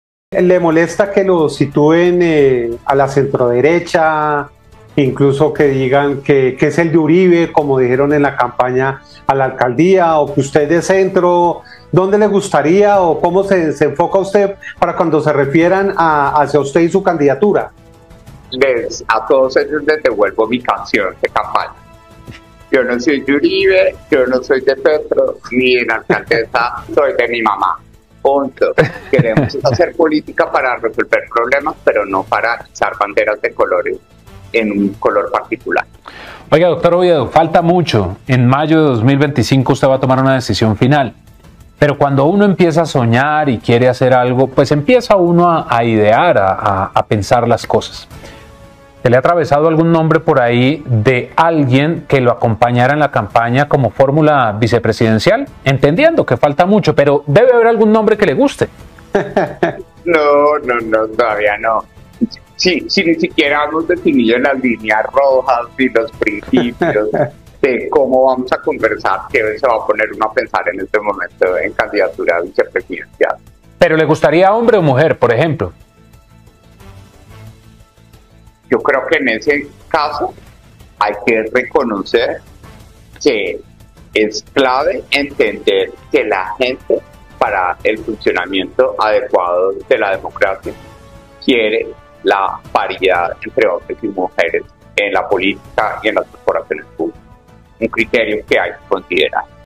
"Yo no soy de Uribe ni de Petro, SOY DE MI MAMÁ: Juan Daniel Oviedo | SEMANA
"Yo no soy de Uribe, ni de Petro, ni de la exalcaldesa -Claudia López-, soy de mi mamá": Juan Daniel Oviedo.